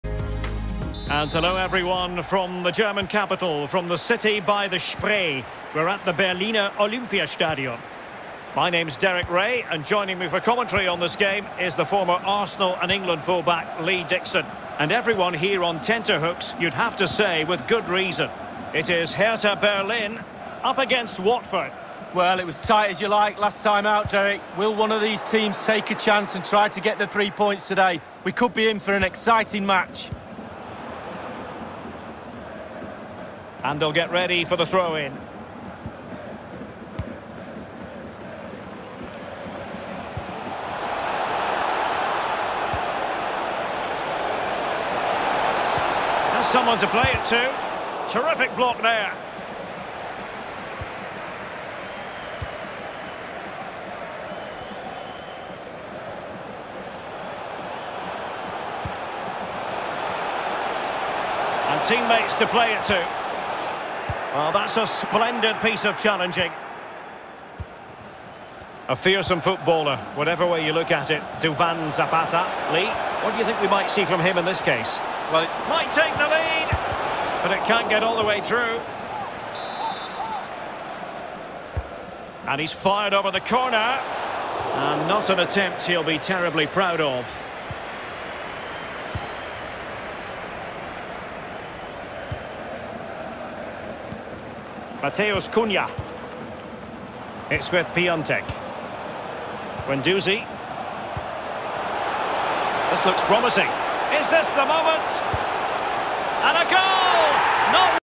(W) Hertha Berlin v Watford (F) - Part Two
FT CWC XV
Round One

Referee: J. Robertford 4/5

M. Cunha (41')
R. Gosens (A)
D. Zapata (56')
M. Cunha (A)
K. Piątek (103')
D. Zapata (A)

W. Hughes (Y)
A. Gray (76')
I. Sarr (A)
I. Sarr (89')
T. Cleverley (A)

MOTM: M. Cunha